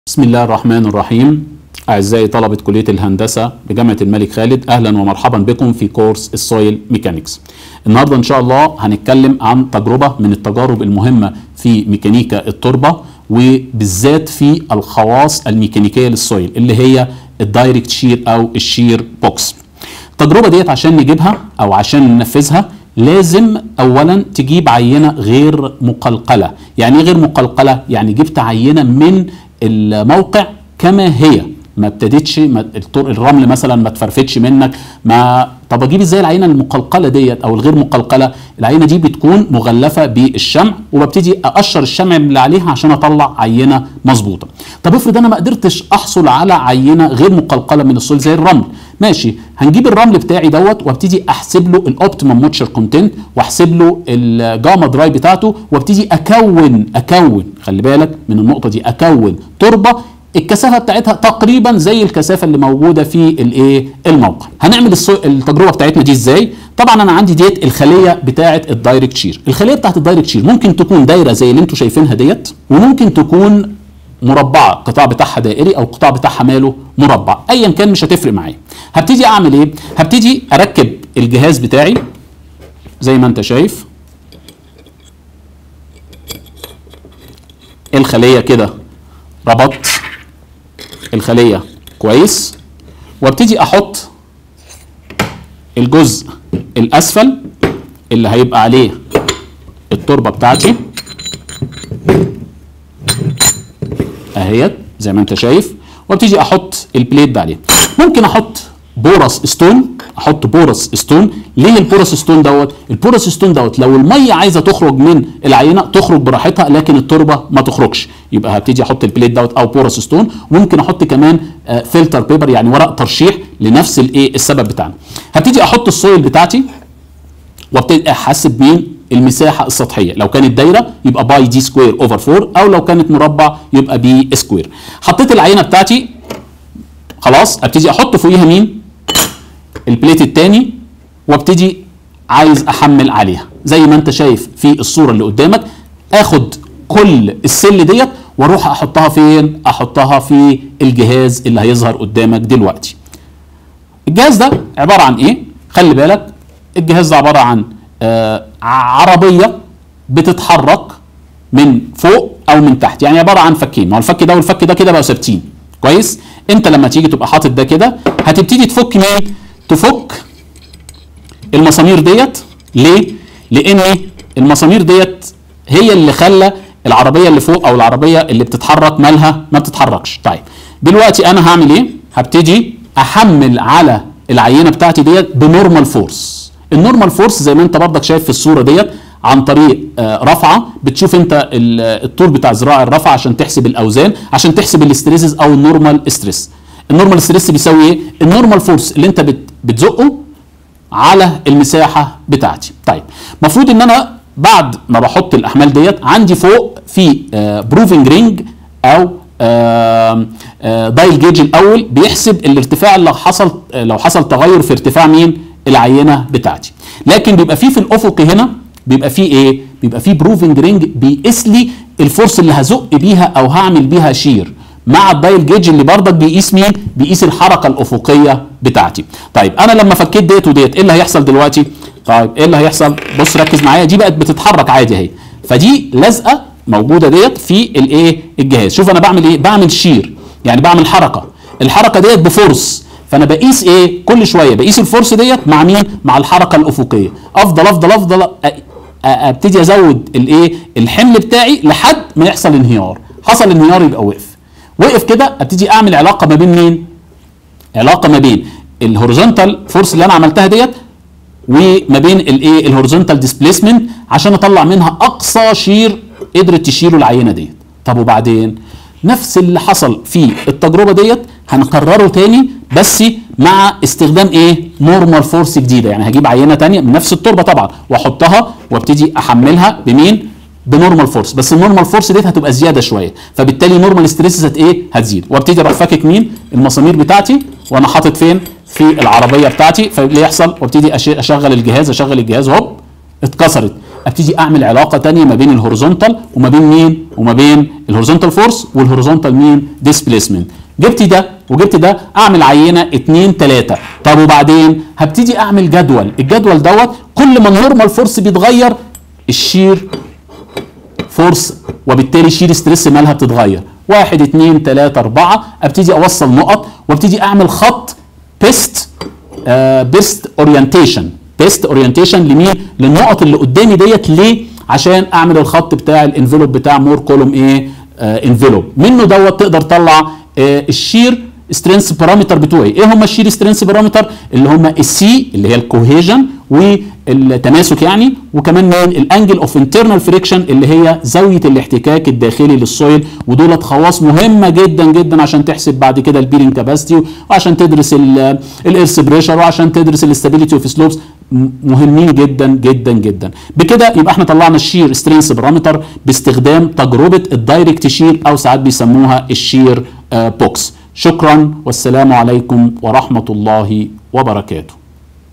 Soil Mechanics ميكانيكا التربة |5-3| Lab3 (shear parameters)
المقررات المفتوحة - ميكانيكا التربة Soil Mechanics
الوحدة 5 : Shear strength of soil
الدرس 3 : Lab3 (shear parameters)
Explain how to perform shear box to get shear strength parameters in the laboratory